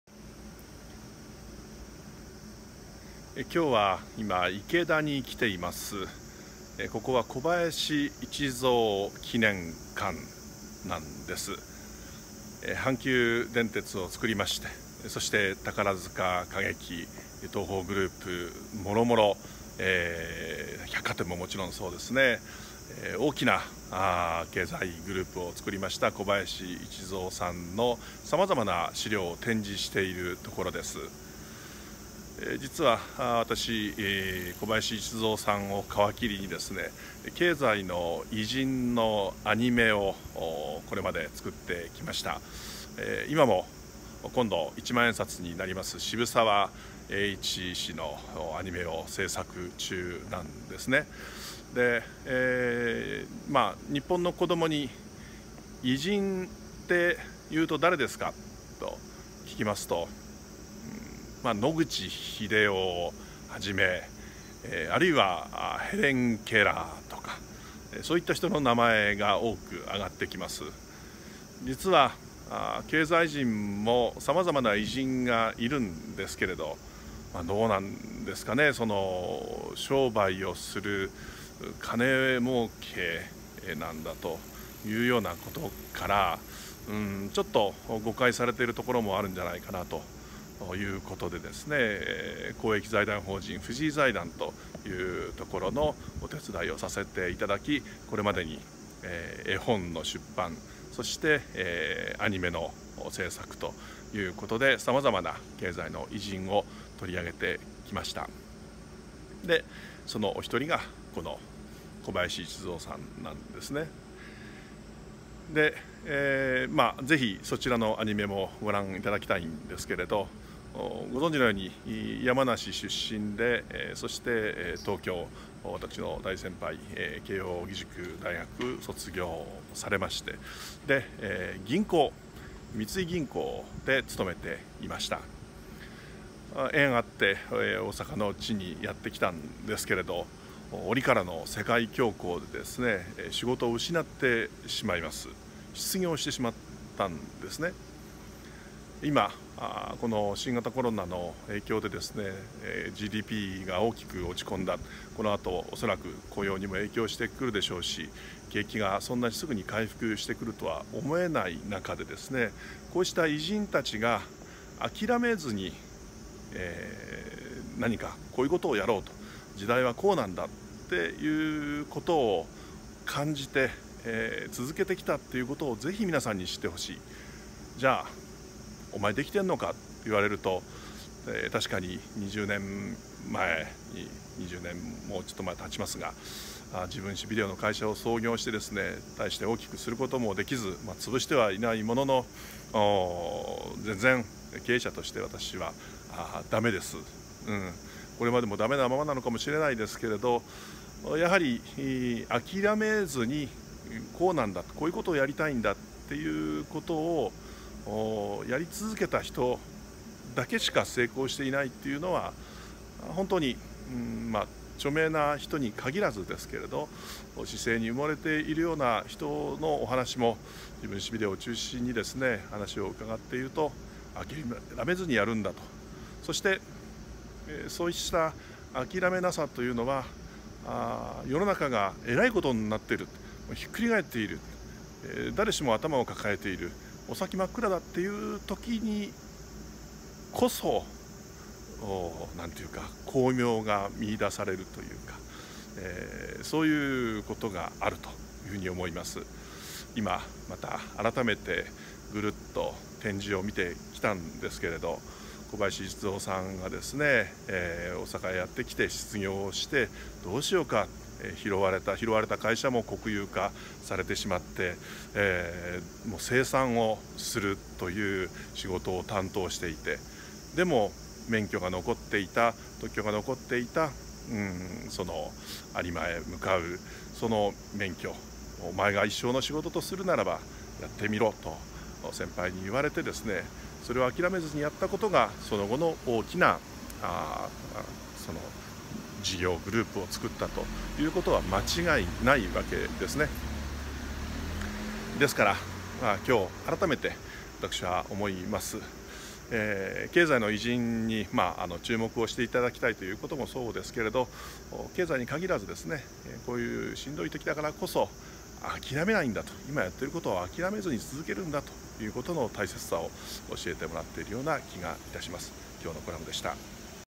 コラム 8/18 経済偉人に学ぶ諦めない心
池田で用事があったので、阪急東宝グループ創設者の小林一三記念館へ行ってきました。

山梨県出身、慶應義塾大学を出て三井銀行に入った小林一三氏ですが、関西に移り住んでいきなり世界恐慌の影響で失業してしまいます。その後の人生については私がプロデュースしたアニメ「小林一三物語」をご覧ください。

失敗しても失敗しても諦めなかった人としては、チキンラーメン発明者の安藤百福氏、二輪メーカーから自動車メーカーとした本田宗一郎氏、パナソニック創業者松下幸之助氏といった経済偉人を取り上げて、アニメを作ってきました。

他にも絵本を読み聞かせる作品として、京セラ創業者の稲盛和夫氏、宅急便を作った小倉昌男氏もあります。

コロナ禍にある今だからこそ、彼らの生き様をお伝えしたいのです。

公益財団法人 藤井財団のアニメはこちらでご覧ください💁‍♂️